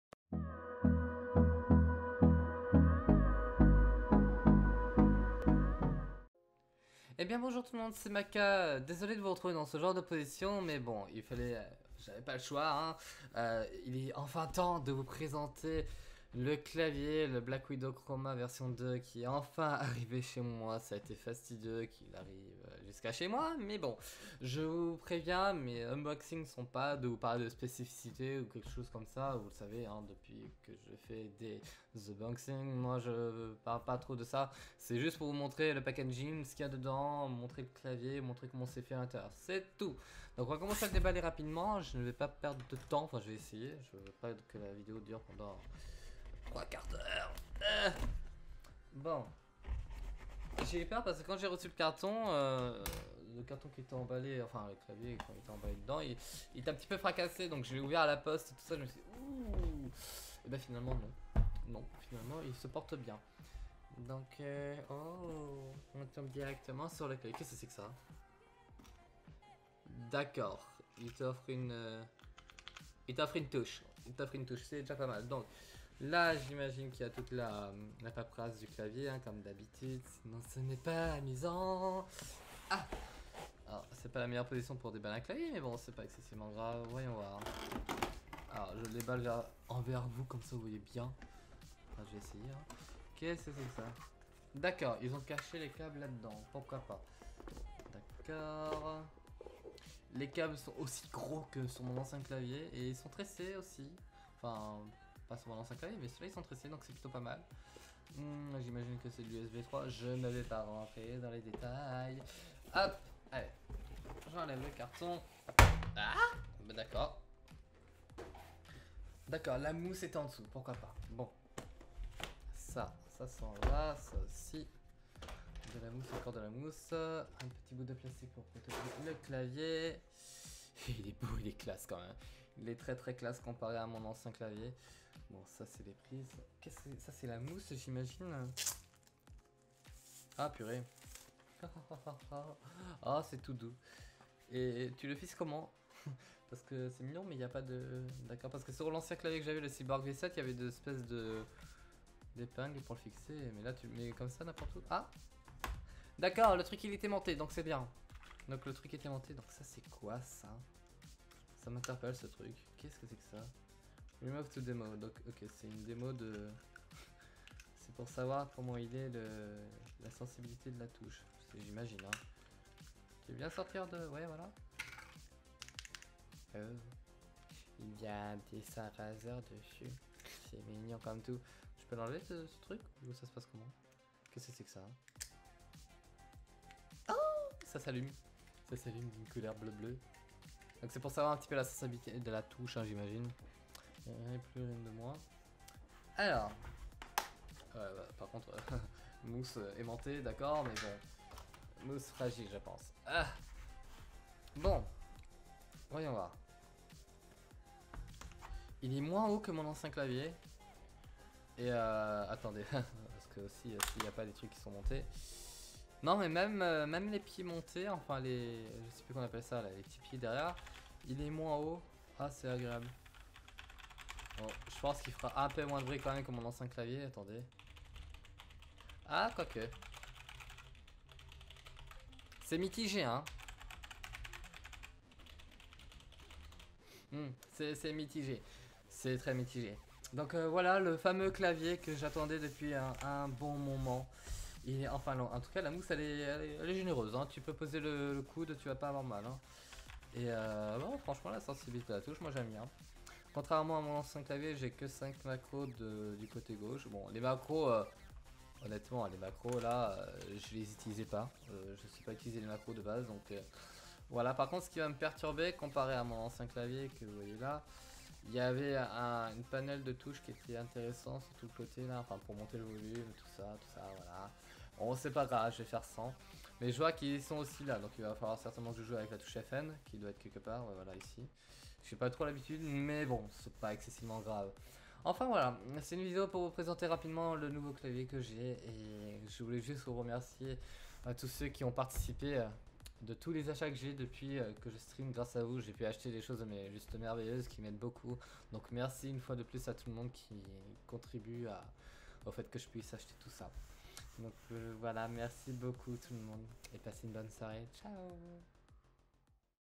[UNBOXING] - RAZER BLACK WIDOW V2 !
Hello les gens ! =)

Enfin la petite vidéo du nouveau clavier ! =)
il était temps ! xD